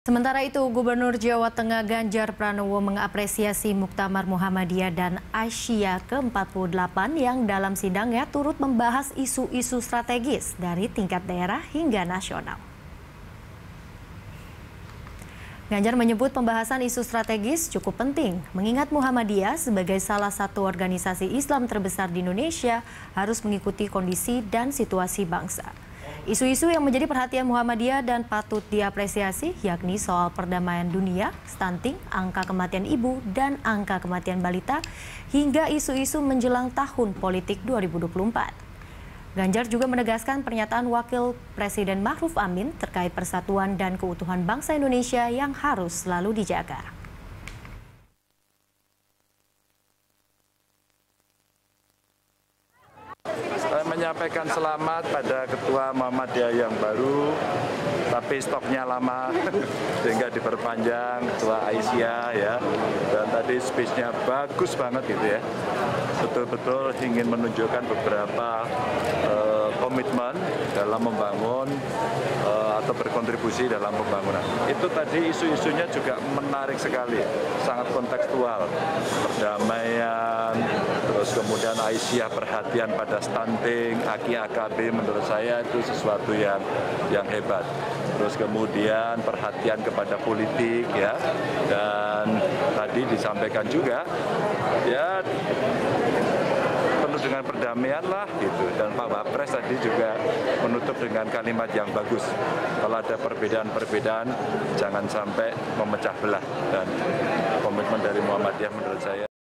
Ganjar Pranowo Apresiasi Muktamar Muhammadiyah d Solo
Gubernur Jawa Tengah Ganjar Pranowo mengapresiasi Muktamar Muhammadiyah dan Aisyiyah ke-48 di Stadion Manahan Solo, Jateng. Di mana dalam sidangnya turut membahas isu-isu strategis dari tingkat daerah hingga nasional.